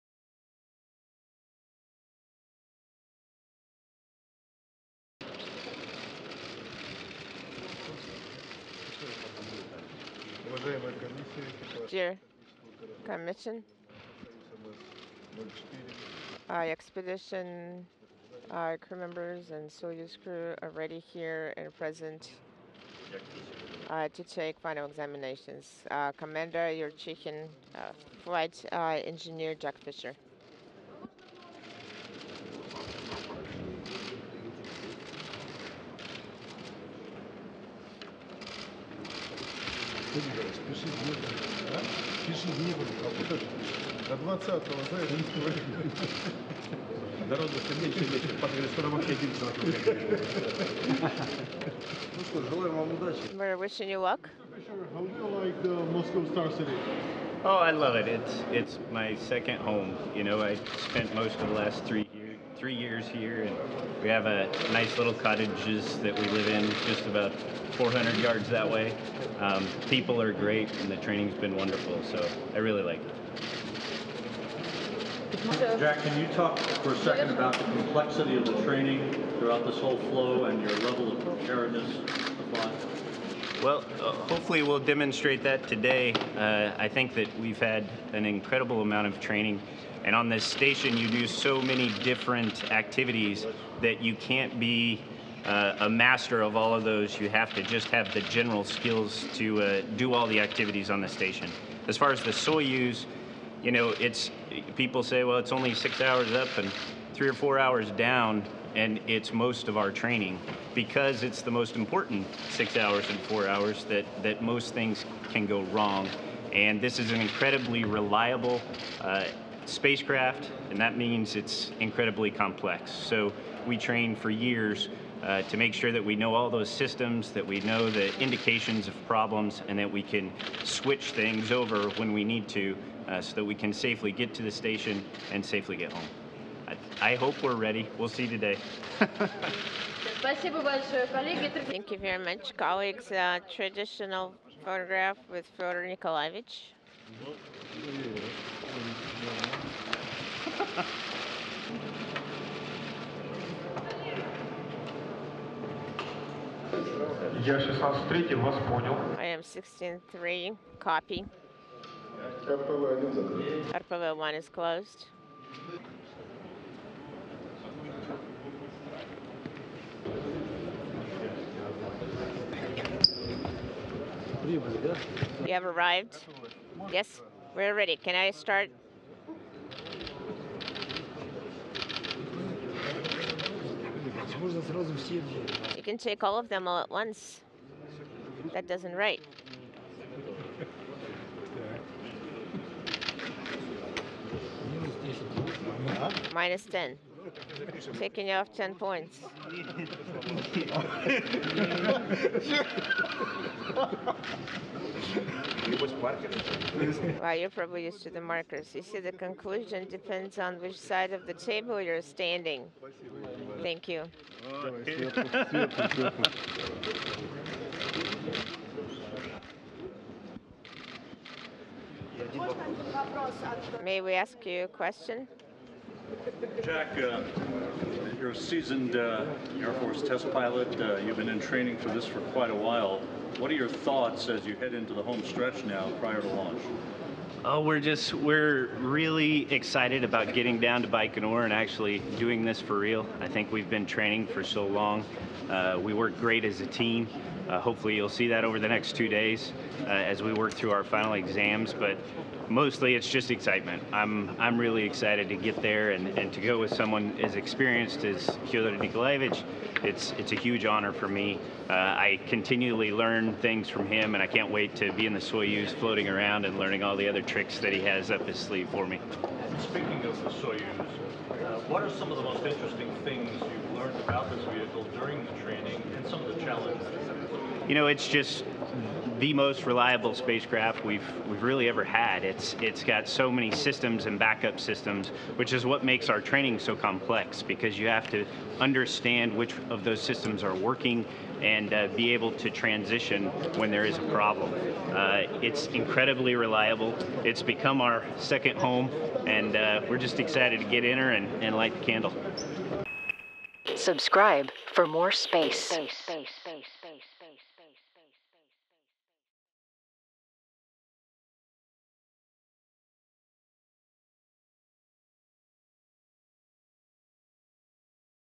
YT Exp 51 Russia Feed Crew Qual Exams
Expedition 51-52 Soyuz Commander Fyodor Yurchikhin of Roscosmos and Flight Engineer Jack Fischer of NASA conducted final qualification training at the Gagarin Cosmonaut Training Center in Star City, Russia March 30 and 31.  Yurchikhin and Fischer are scheduled to launch on April 20 from the Baikonur Cosmodrome in Kazakhstan in the Soyuz MS-04 spacecraft for a four and a half month mission on the International Space Station.